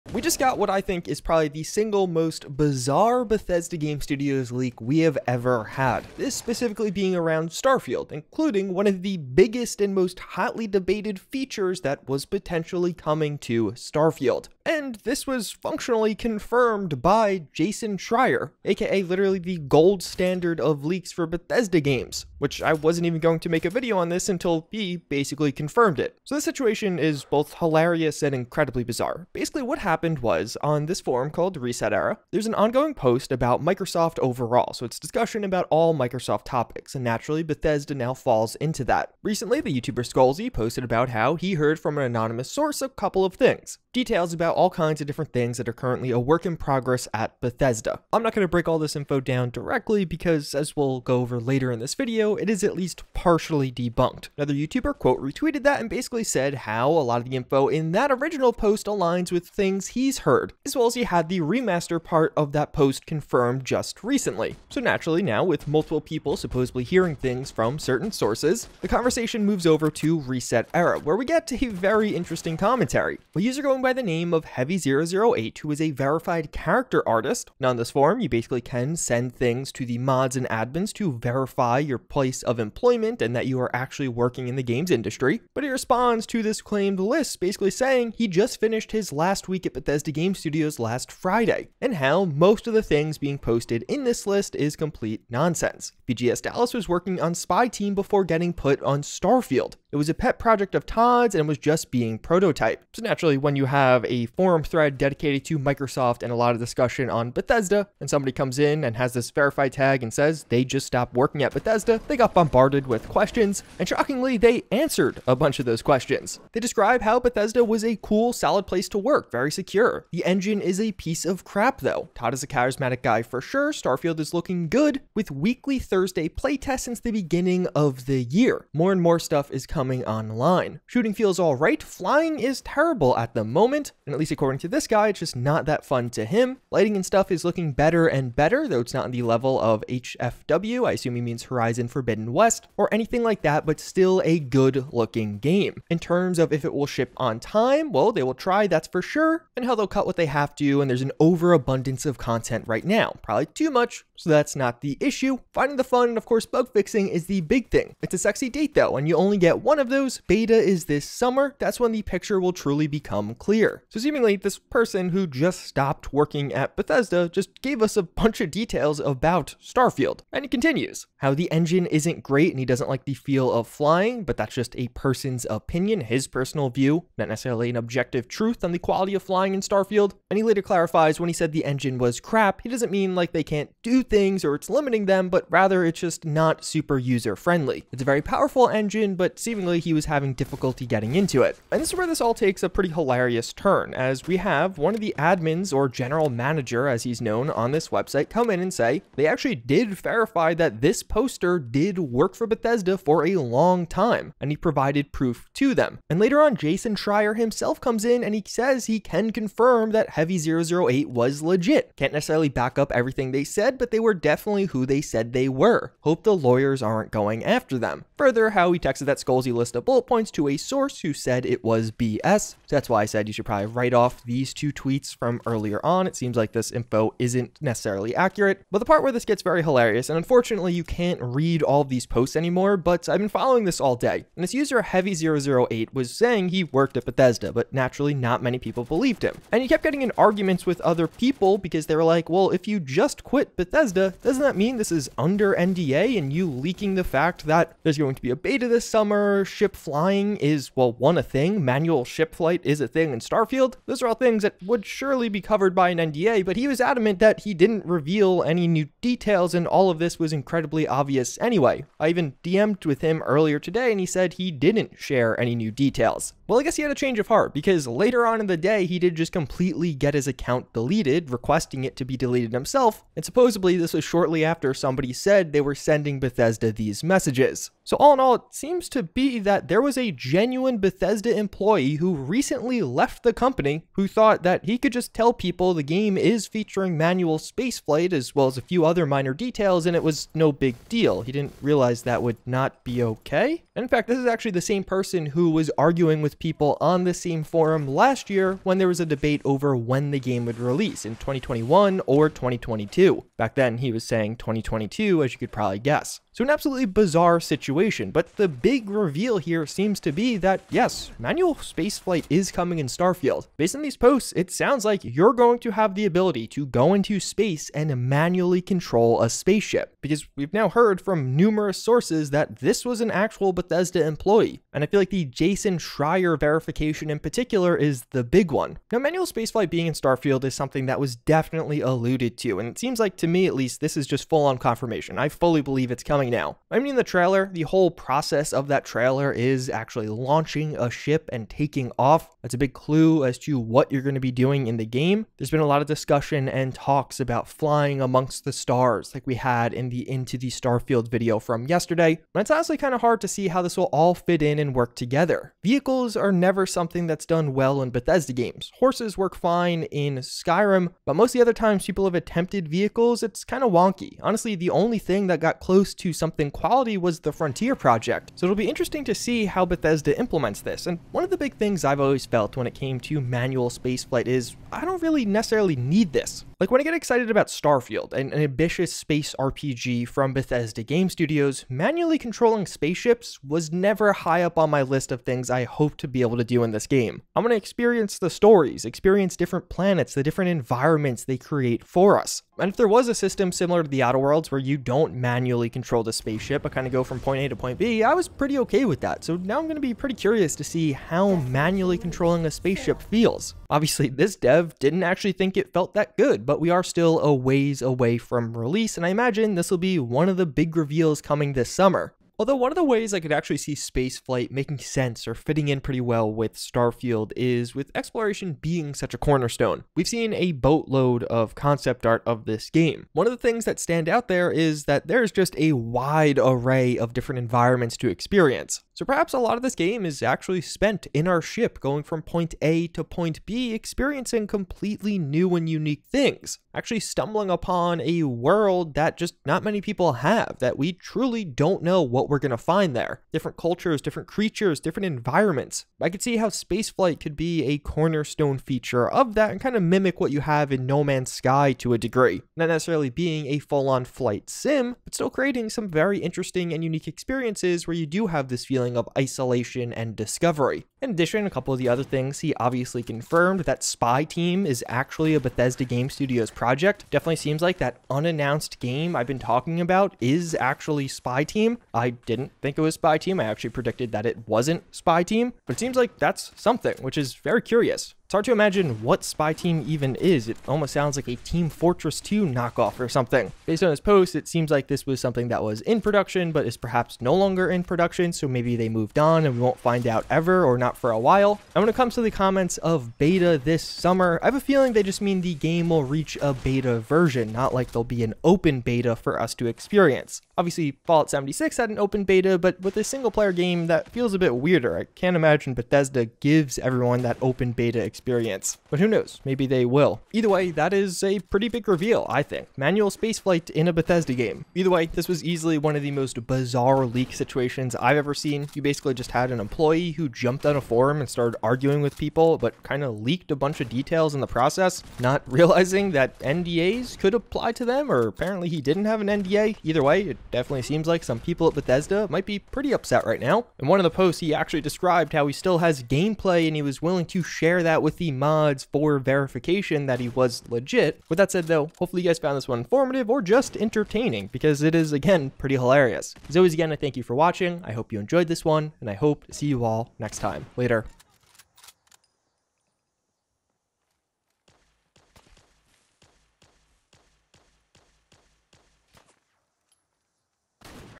The Starfield Leaks Are Getting INSANE
Today we look at the latest Starfield leak which seems to confirm that manual space flight is coming to Starfield, as well as some other new details.